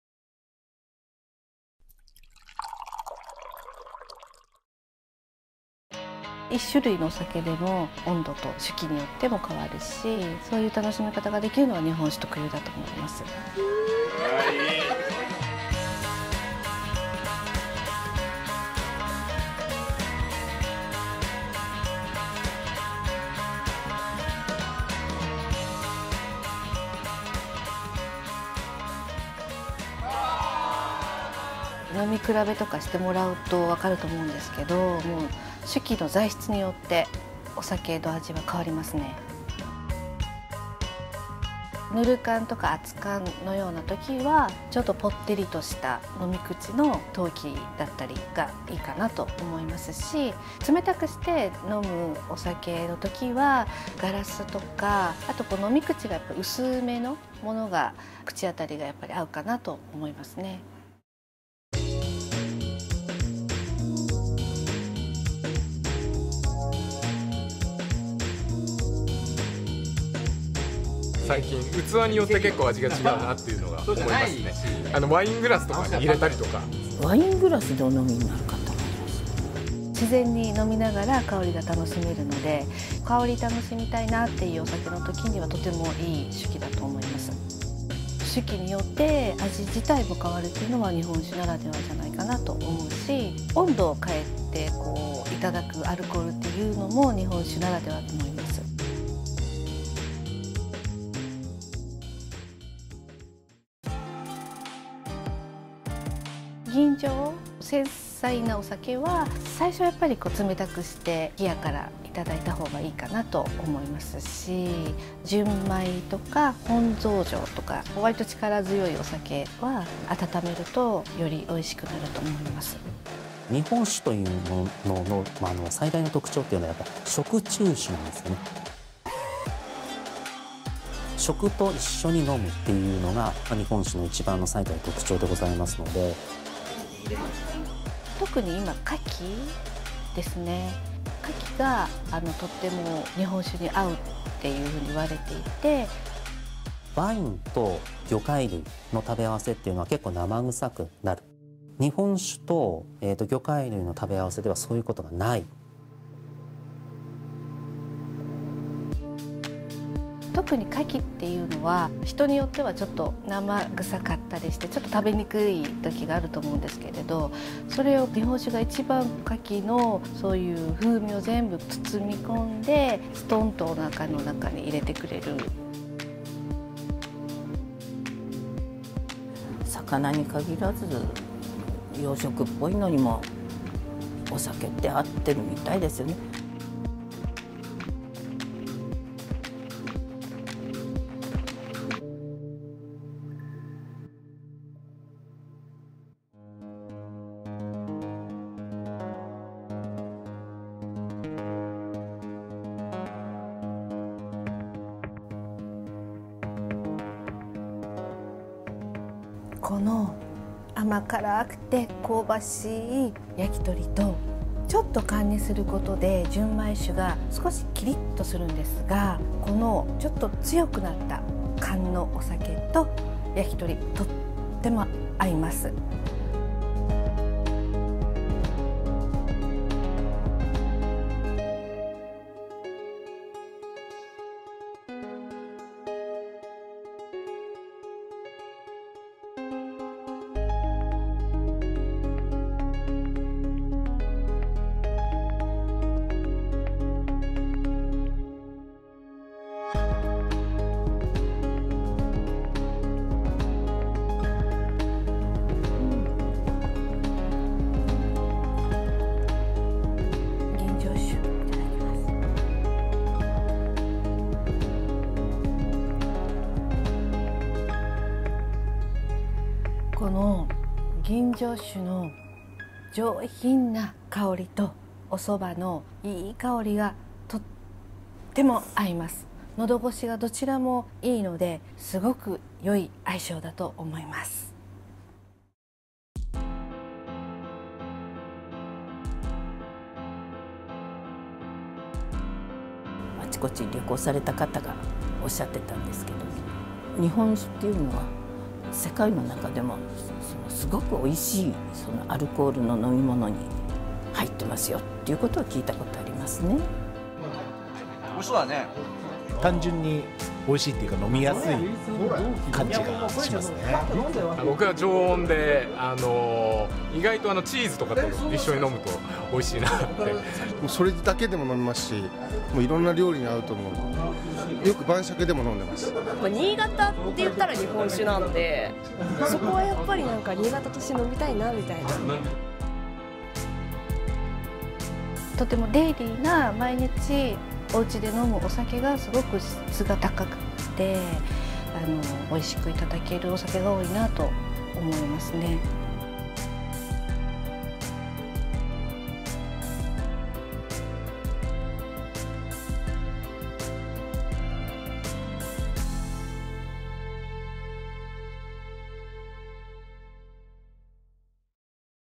【新潟シティチャンネル】NIIGATA SAKE 消費編 「酒器と温度とペアリング、新潟清酒の楽しみ方」【中国語・繁体字版】
"日本一の米どころ新潟の食文化を代表する米と酒を、
海外に向けて発信するプロモーションムービーです。
作り方や食べ方などをテーマごとに分けて紹介しています。"

新潟シティチャンネルでは新潟市の魅力や市の事業を動画で紹介しています。